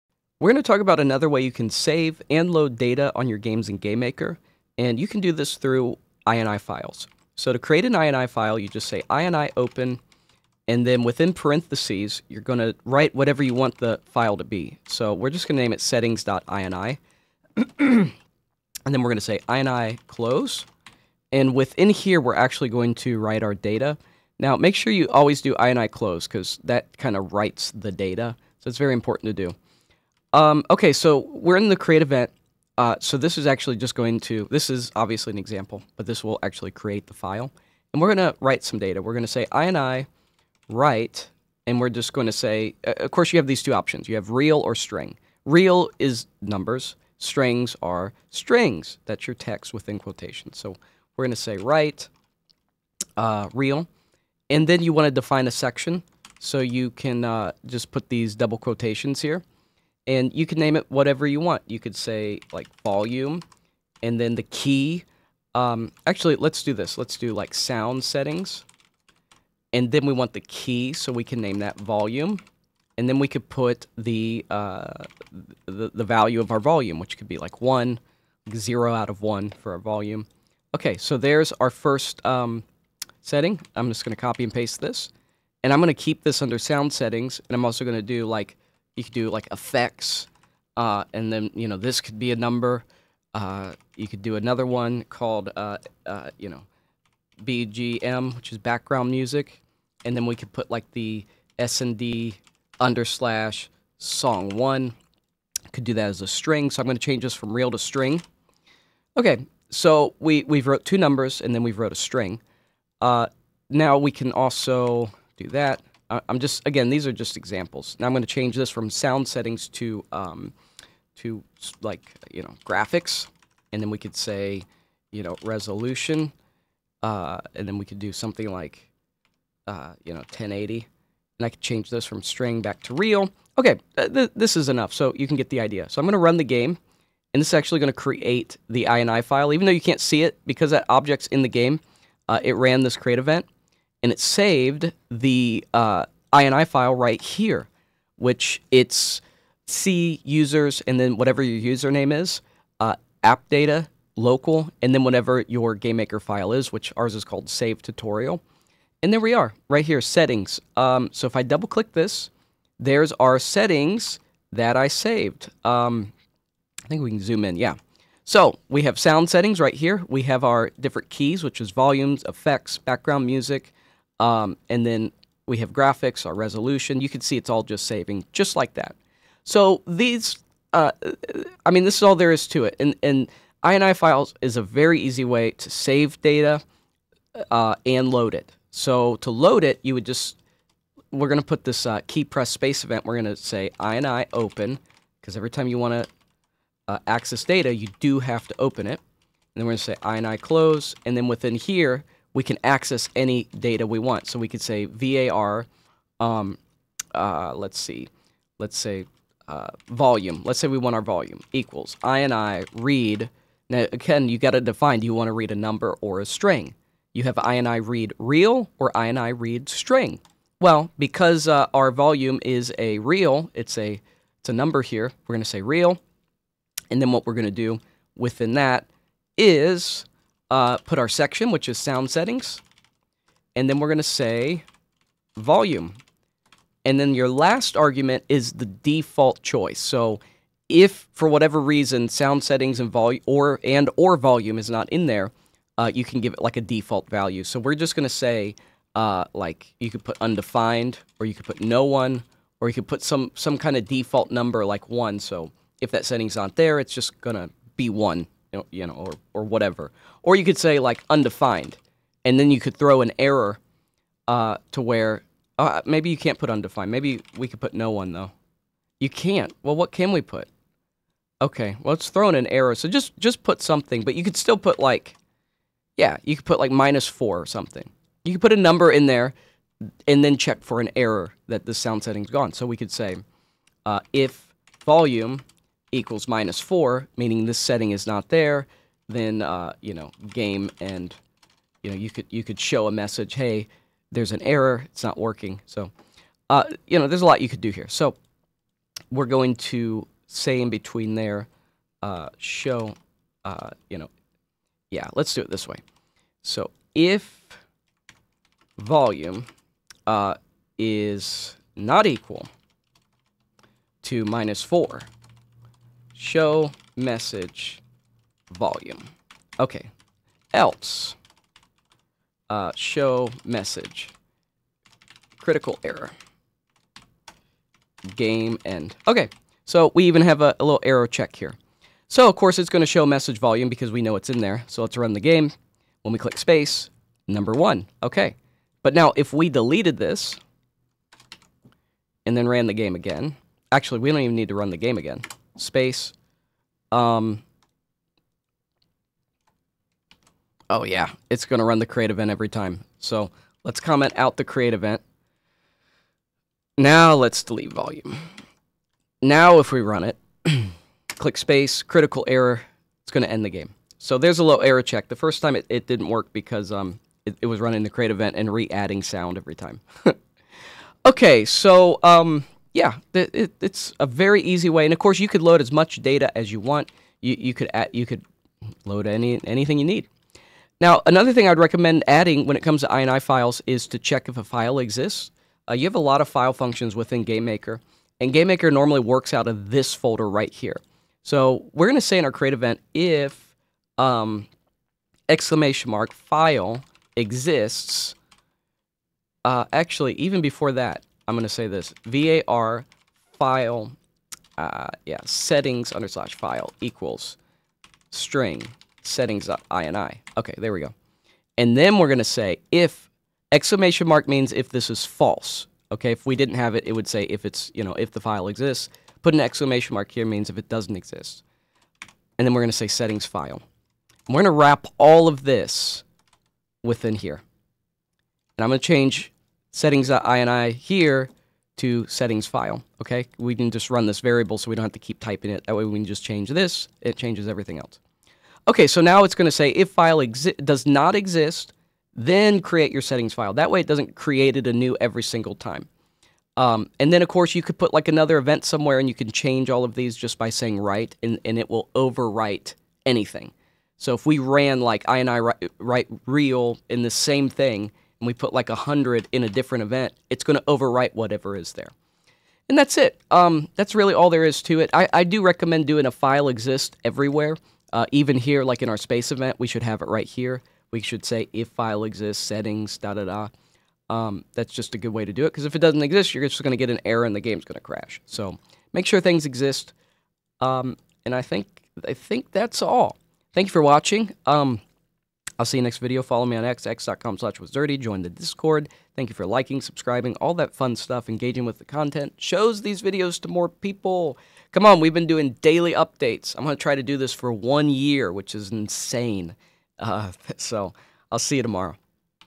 INI Files | Save/Load Settings & More | Beginner Tutorial
An INI file in GameMaker's GML is a simple text-based configuration file used to store and retrieve structured data, such as settings, player preferences, or game progress, using key-value pairs within named sections. It allows for easy reading and writing of persistent data with functions like ini_open(), ini_write_string(), and ini_close().